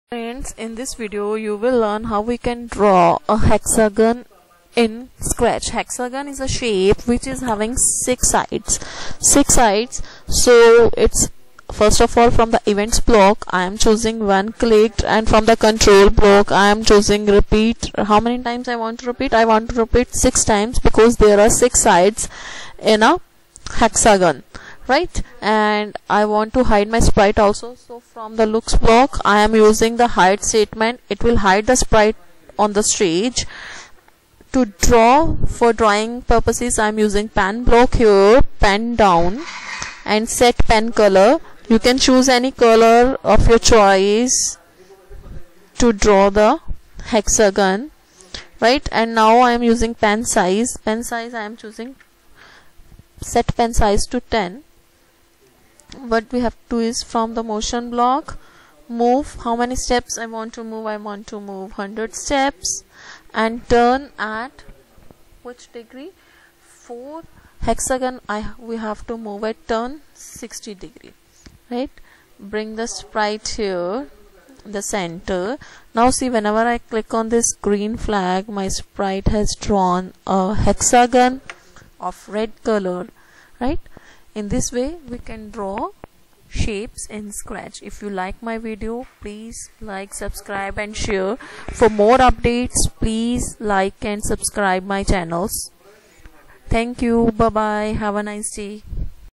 DRAW A HEXAGON IN SCRATCH | latest computer videos | Latest IT videos | 2022
In this video,  you will learn to draw a SHAPE i.e HEXAGON in Scratch. We used various blocks from Motion Block Menu, Control Block Menu, Look Block Menu and Pen Block Menu. Hide block is used here from LOOK block Menu which hides the sprite from the stage while working.  So let us learn how to drawHEXAGON in Scratch.